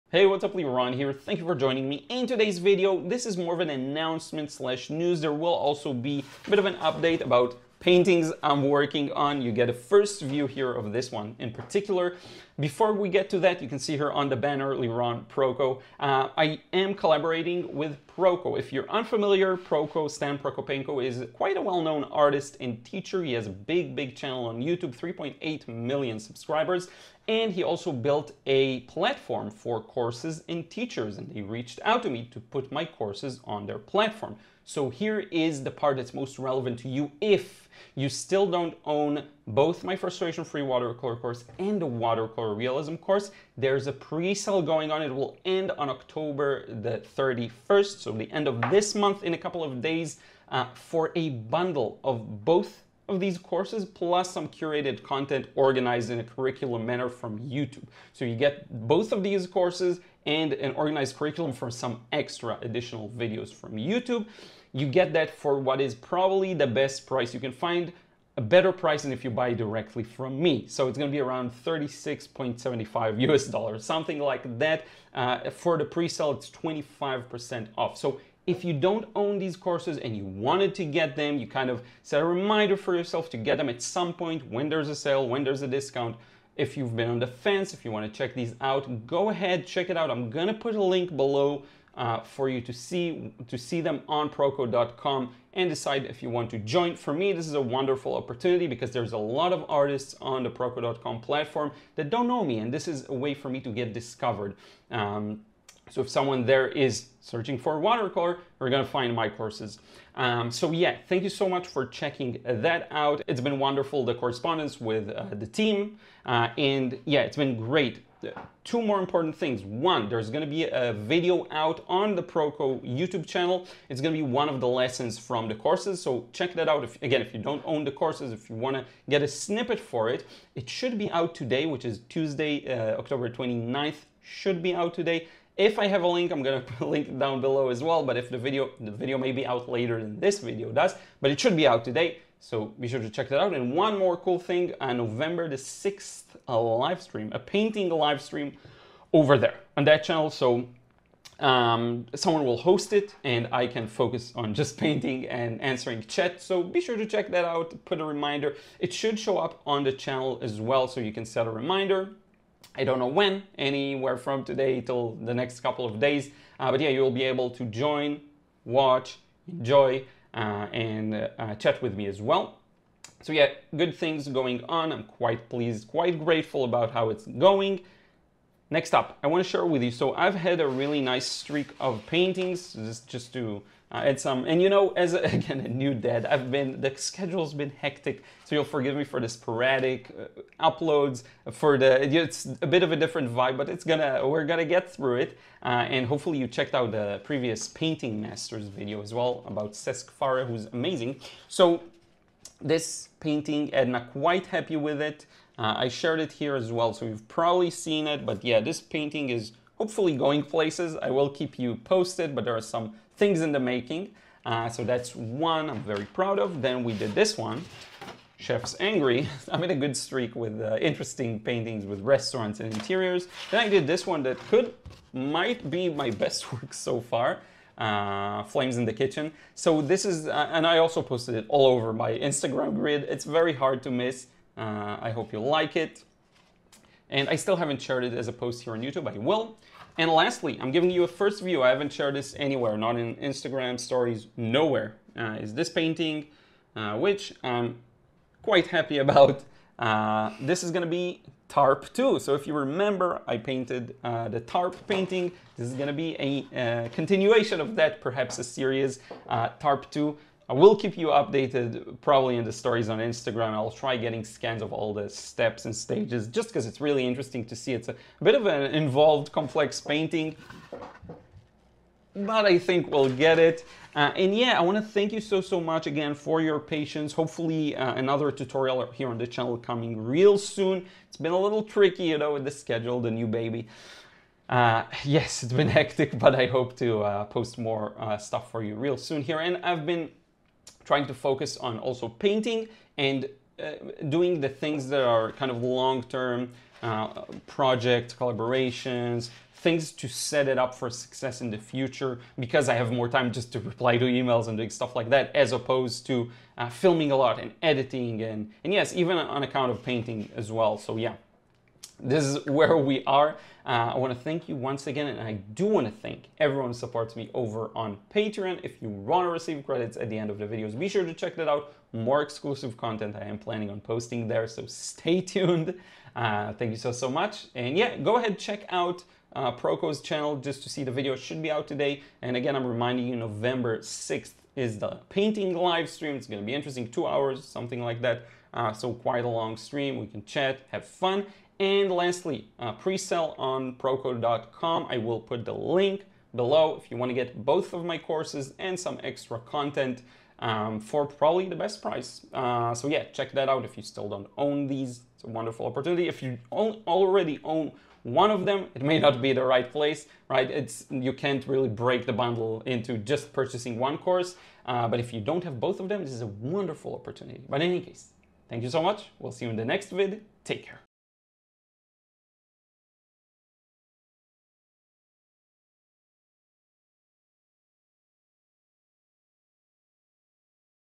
Liron x Proko Collab & New Paintings
Today I wanted to briefly update you regarding my Watercolor Fundamentals Bundle on the Proko website...

As well as some paintings I just completed, and a new painting I'm currently working on.

If you still don't own my FFW course and WR course, be sure to check out the Watercolor Fundamentals Bundle here:

I also want to thank the many people who have already messaged and emailed me, letting me know they joined 🙏😊

- Liron

My recommendations are always based on my own personal experience ^_^